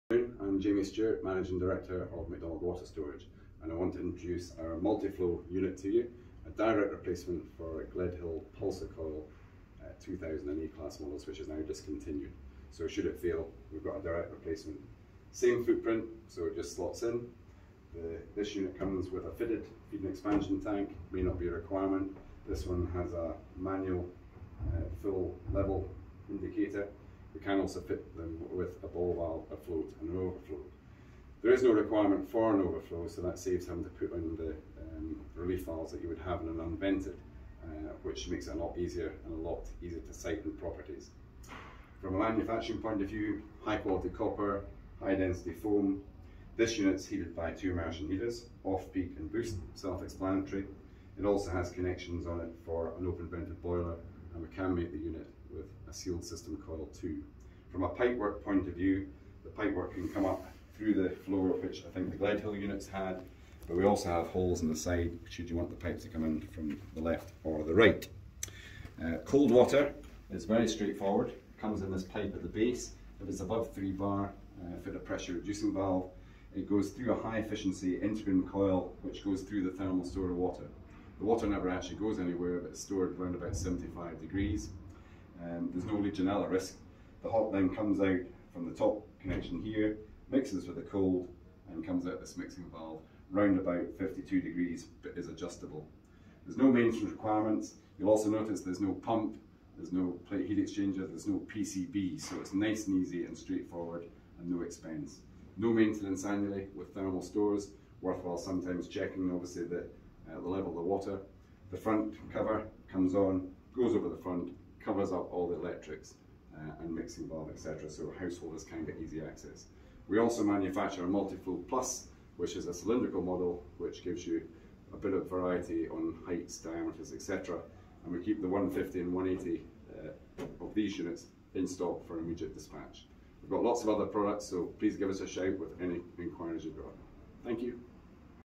MULTIflow Overview
Our MULTIflow thermal store range is specifically designed for the contract market, to provide a high performance, mains pressure hot water solution which delivers enormous benefits.

Our thermal store approach can suit multiple installation applications, including electric, boiler or central plant inputs to meet hot water demand. With our unique design and manufacturing solution, we can offer significant cost savings both during installation and ongoing running costs.

Manufactured from premium grade copper, our MULTIflow mains pressure hot water system delivers unrivalled performance thanks, in part, to copper’s superior thermal transfer properties.

The MULTIflow is a direct thermal store, but with the additional flexibility to incorporate a boiler, to provide mains pressure hot water, with two model variants, MULTIflow OV and MULTIflow SB, ranging from 120 to 210 litres.

This multi-application thermal store range can be used with an open vented, sealed or central plant boiler and/or off-peak electricity to aggregate the supply to heat the store, and provide domestic hot water when it is required, keeping running costs for the householder down to a minimum.

The MULTIflow also provides the perfect Gledhill PulsaCoil replacement option, but by incorporating the internal heat exchanger to generate the hot water, there are no costly pumps, controls or extra electrical supplies required.